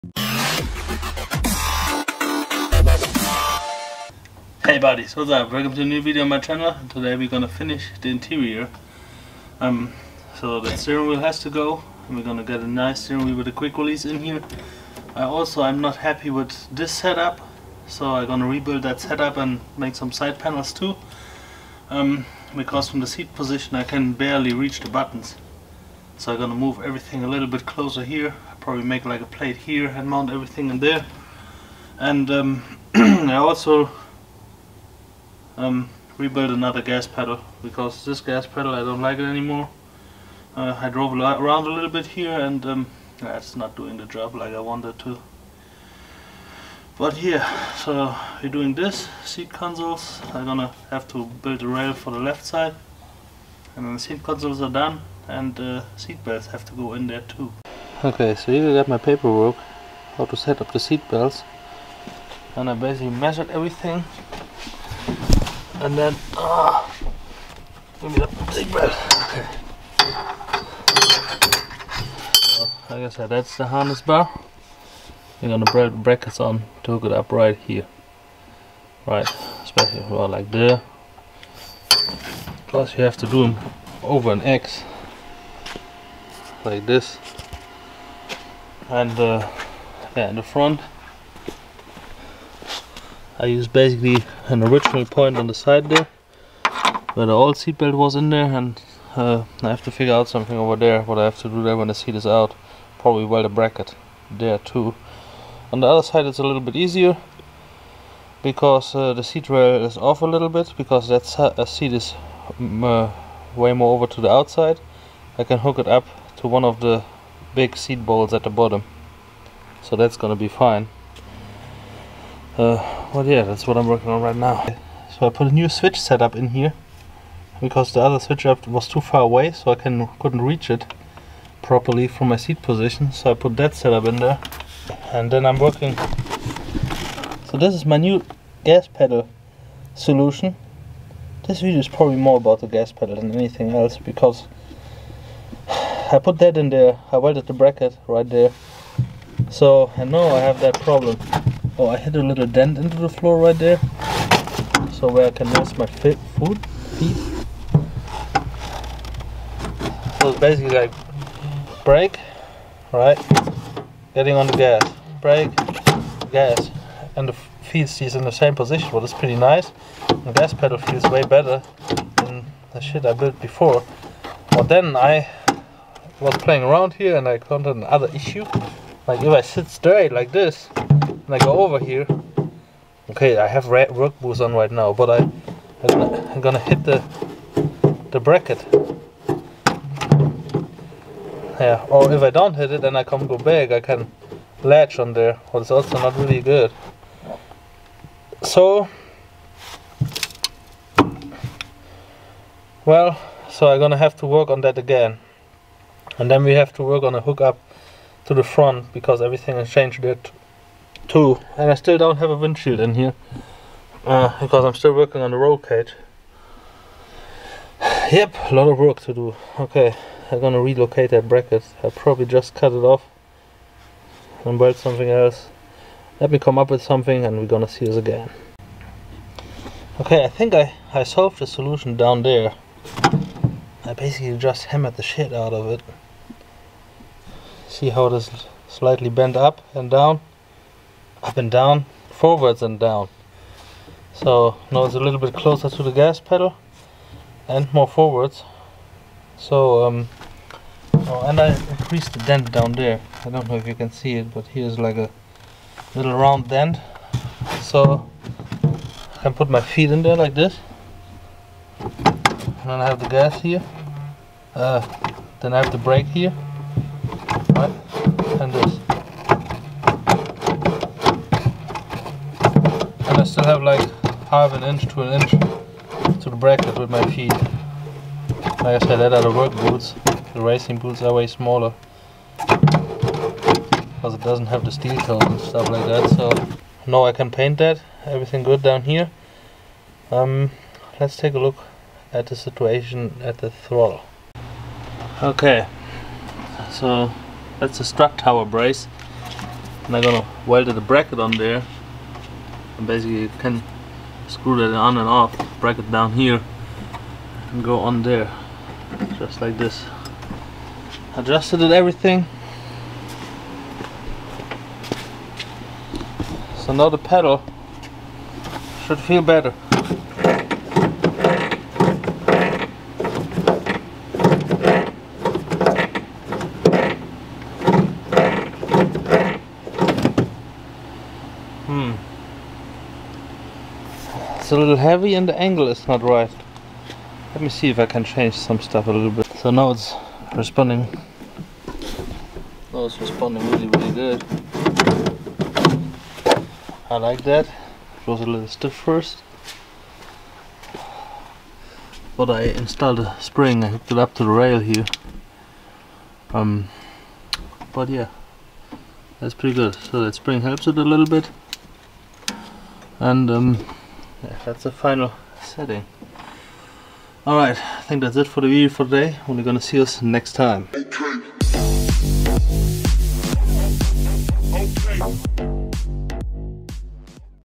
Audi TT MK1 LS Swap interior work the gaspedal trouble continued, can i solve it this time?!
In this episode im working on the interior and build again another gaspedal, this is my 4th gas pedal setup on this car!